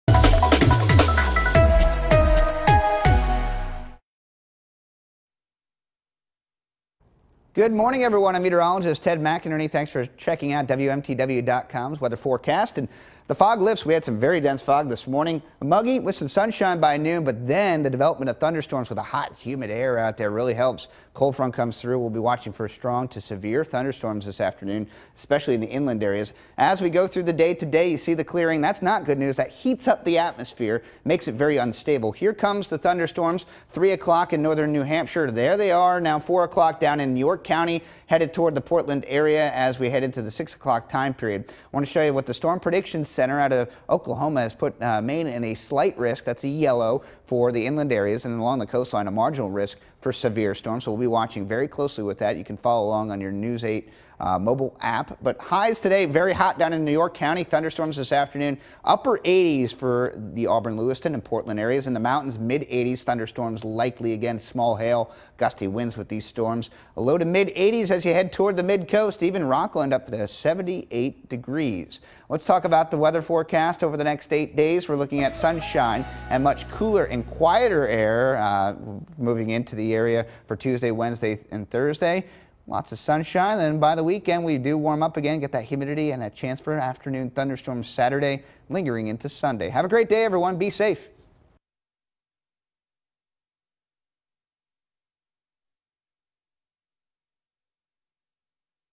Ted's Monday morning forecast
How’s the weather looking for Monday morning? Get your latest Maine’s Total Weather video forecast from Meteorologist Ted McInerney.